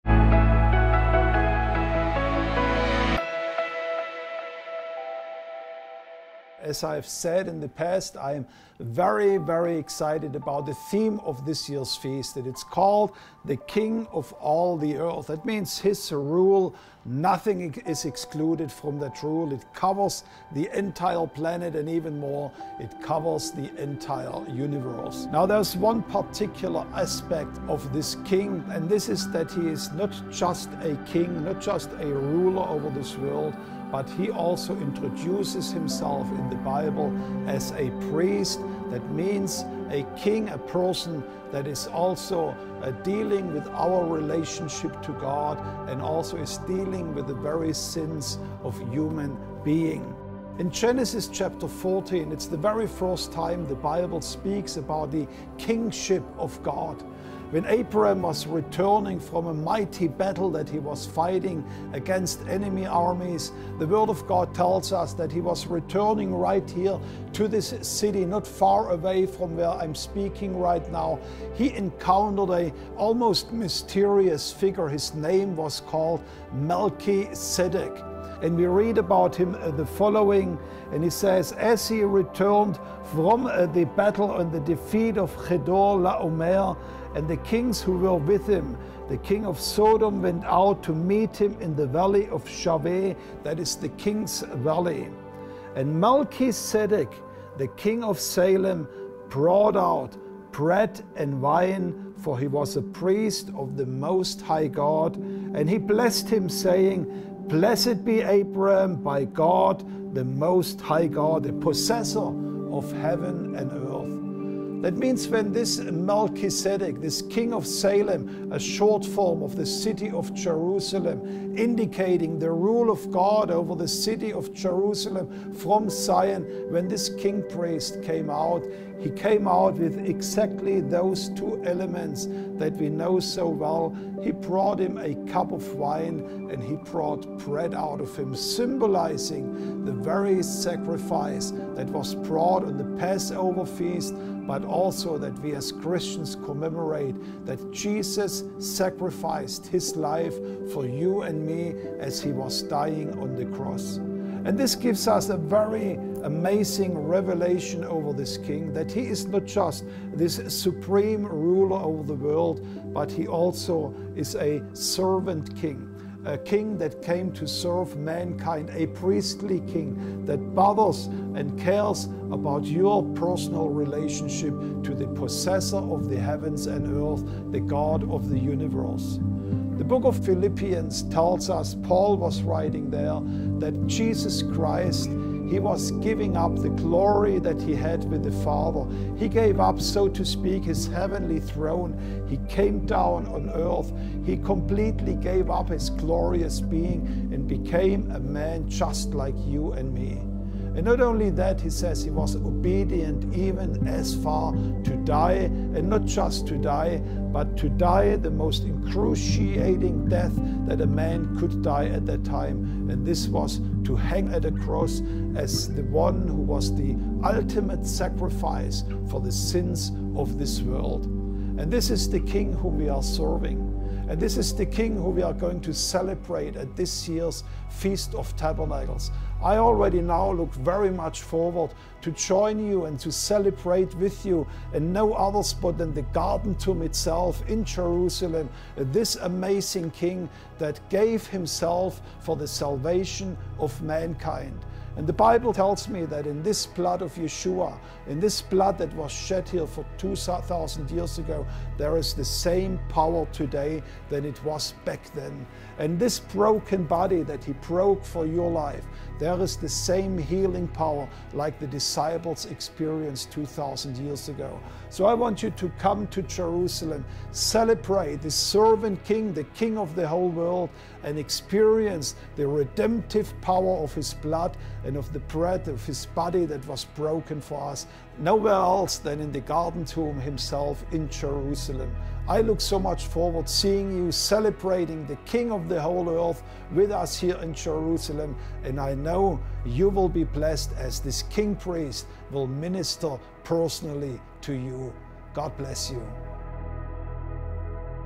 Feast of Tabernacles Ep 5 - King as High Priest
Join us in the final episode, where we look at God as the Most High Priest - Who atones for every sin and cleanses us from all unrighteousness. The 'Malchitzedek' Who acts as the Servant King, the ultimate sacrifice the world needed, and the One Who cares about our relationship to the God of the universe.